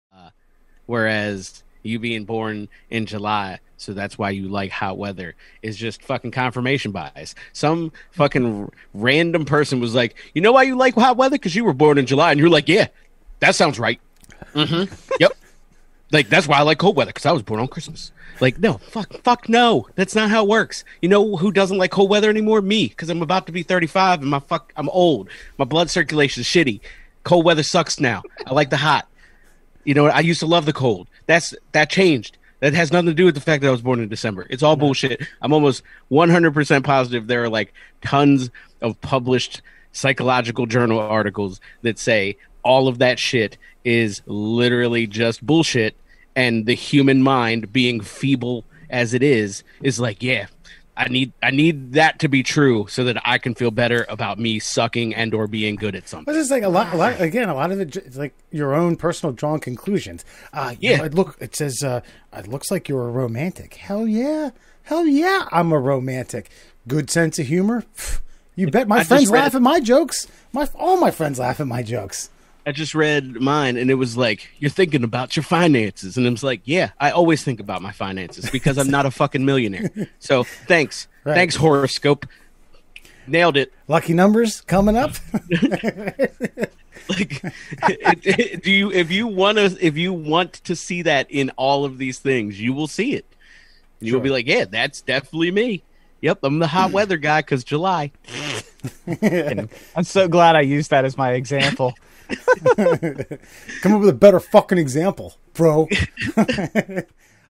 Horoscopes
Alan explains that they are 100% pure BS.